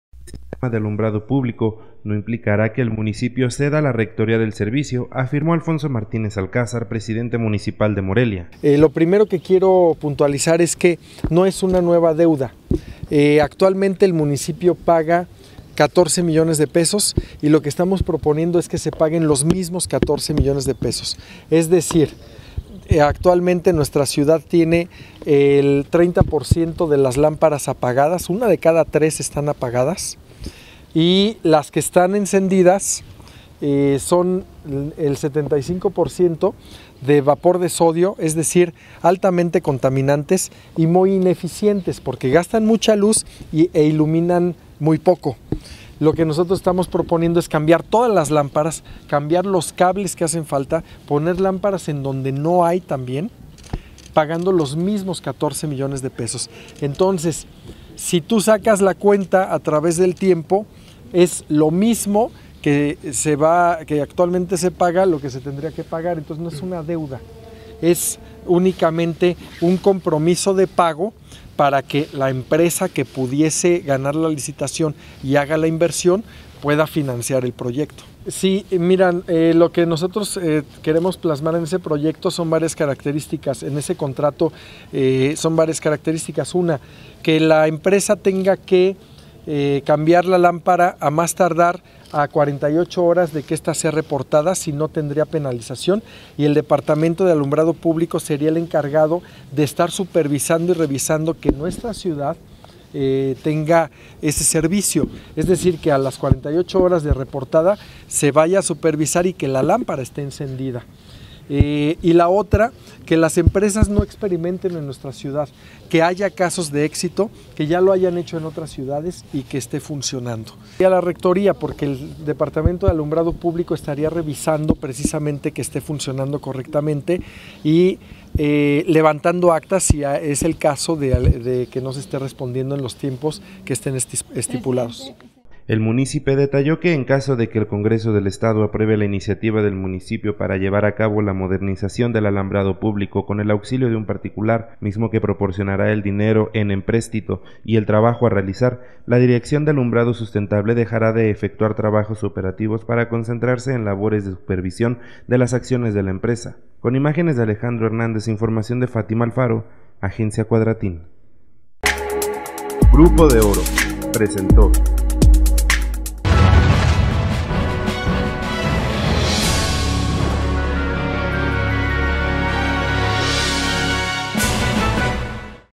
Proyecto de luminarias no conllevará a privatización del servicio
MORELIA, Mich., 12 de julio de 2017.- La configuración de una asociación público privada (APP) para la modernización del sistema de alumbrado público no implicará que el municipio ceda la rectoría del servicio, afirmó Alfonso Martínez Alcázar, presidente municipal de Morelia.